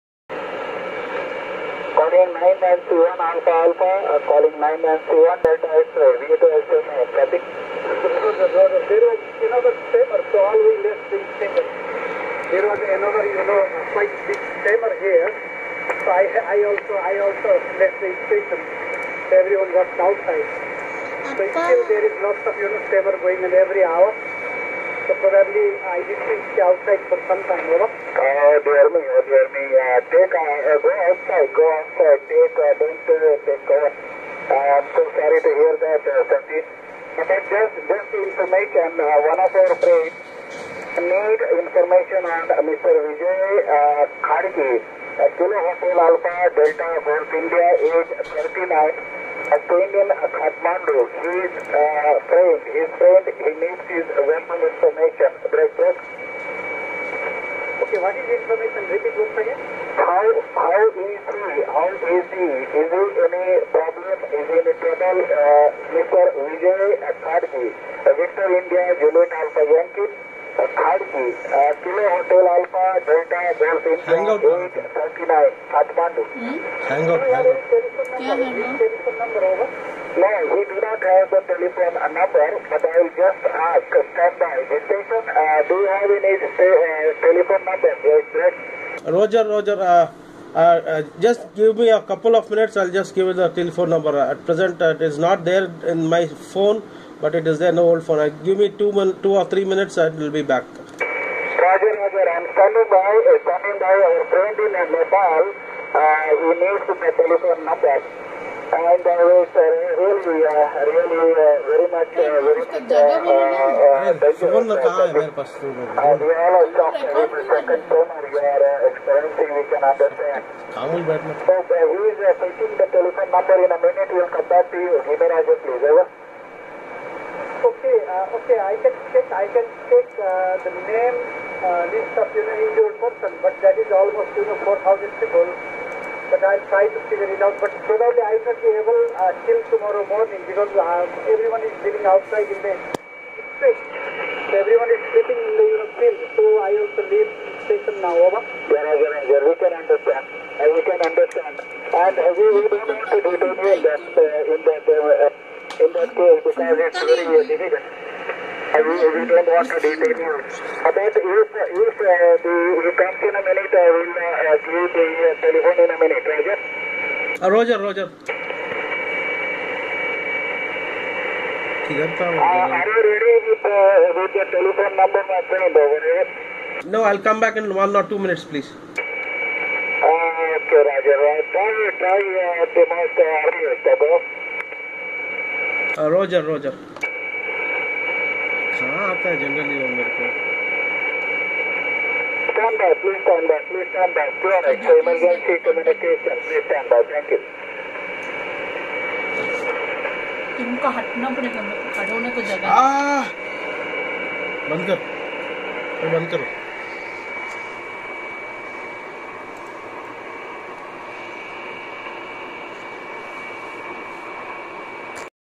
HF NEPAL Emergency COVERSATION
NEPAL Emergency - Amateur Radio Stations helping at earthquake zone